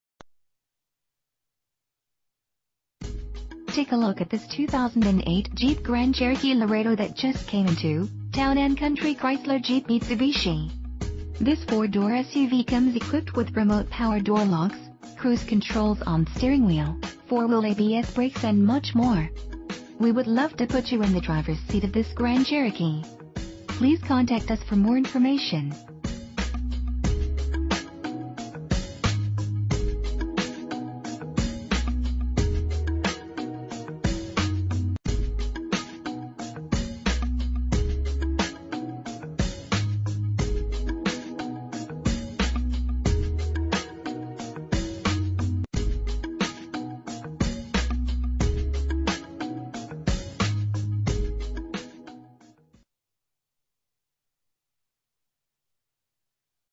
2008 Jeep Grand Cherokee - Milwaukie OR
Year: 2008
Make: Jeep
Model: Grand Cherokee
Engine: 3.7 liter 6 cylinder 12 valve
Transmission: 5-Speed Automatic
Exterior: Black
Miles: 36573

Town & Country Chrysler-J